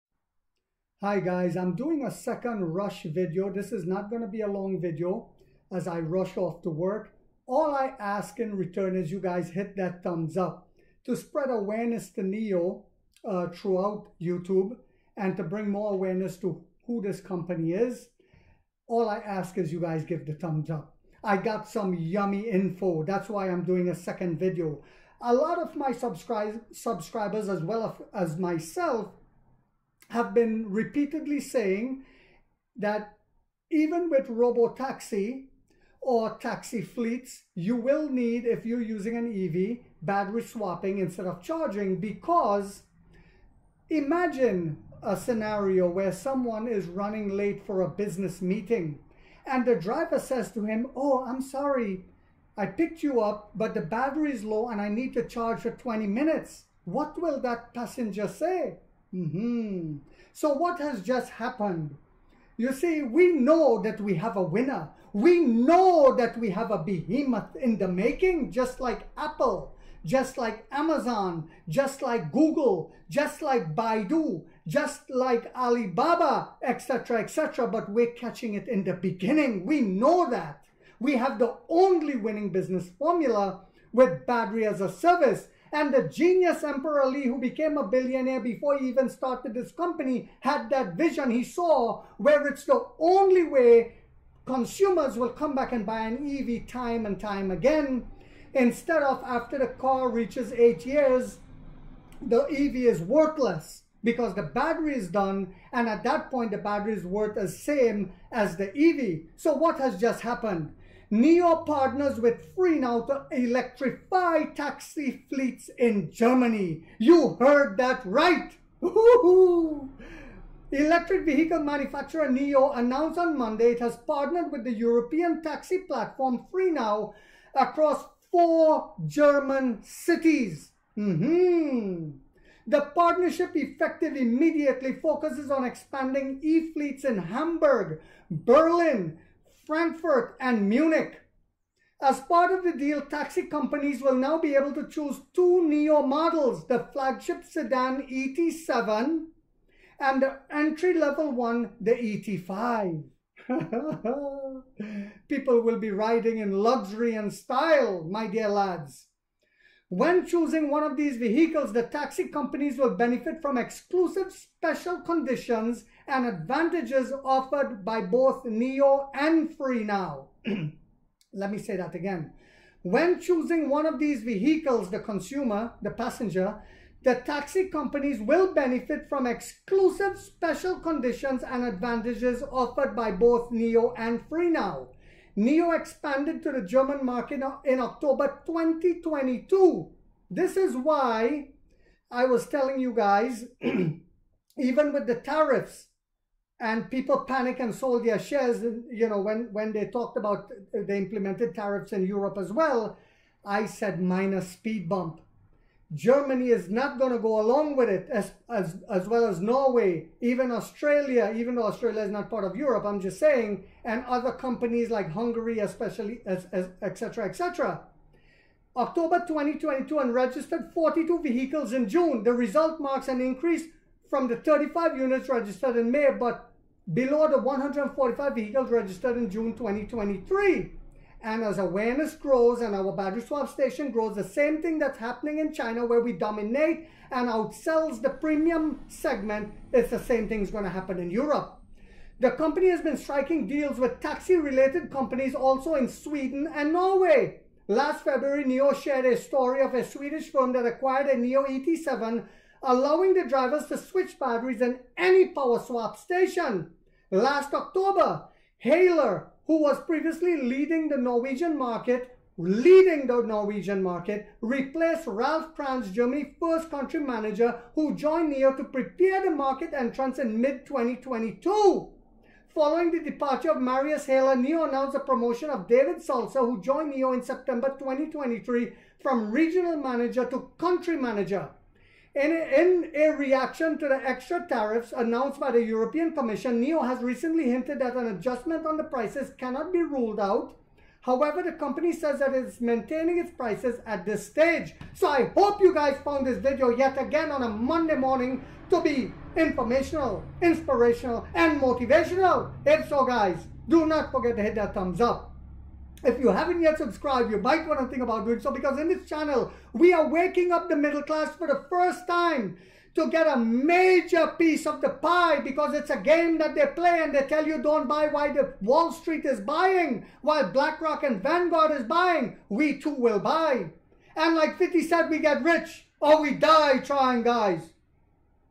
We Were Right European Tariffs Is Not Stopping Nio Taxi
Nio is the leader in the premium luxury electric vehicle space in China